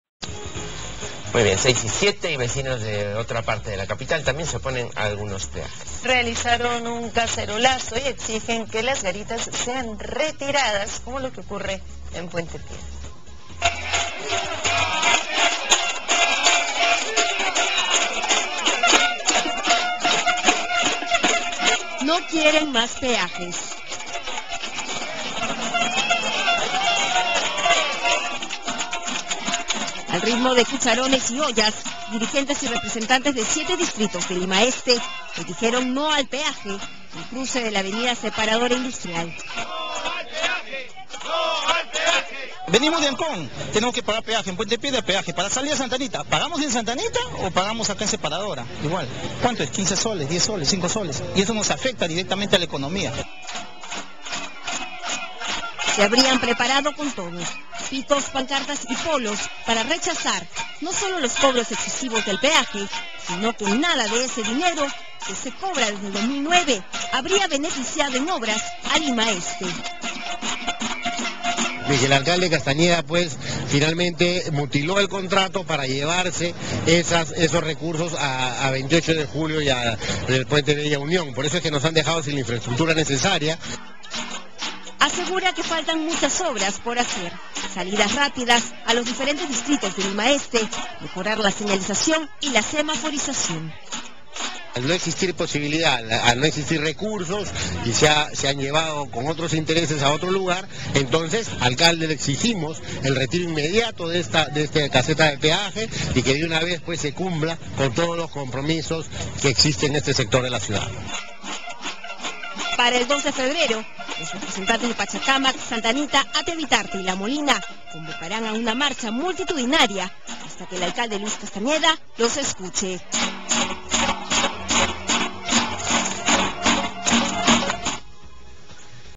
Vecinos de La Molina pidieron retiro de peaje de avenida Separadora Industrial
Vecinos de los distritos de La Molina, Ate y Santa Anita realizaron una protesta en la que exigieron el retiro del peaje ubicado en la avenida Separadora Industrial considerando que es un tramo corto y en el que señalan que Lamsac no le hace mantenimiento adecuado. Ante ello el alcalde de La Molina Juan Carlos Zurek señaló que el alcalde de Lima no acudió a la mesa de diálogo que busca darle solución al tema.